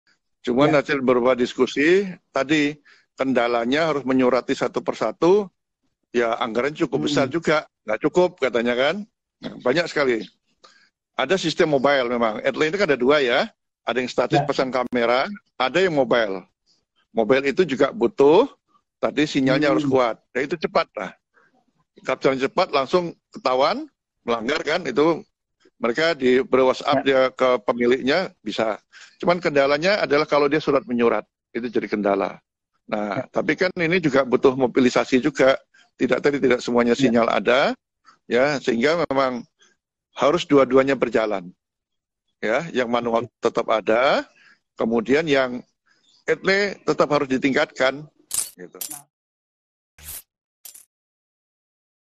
ANGGARAN PENGIRIMAN SURAT TILANG DISEBUT JADI KENDALA PENERAPAN ETLE
Wakil Ketua Bidang Pemberdayaan dan Penguatan Kewilayahan MTI, Djoko Setijowarno menyebutkan kendala yang dialami saat penerapan Electronic Traffic Law Enforcement (E-TLE) atau tilang elektronik. Salah satunya, anggaran pengiriman surat konfirmasi yang terbatas.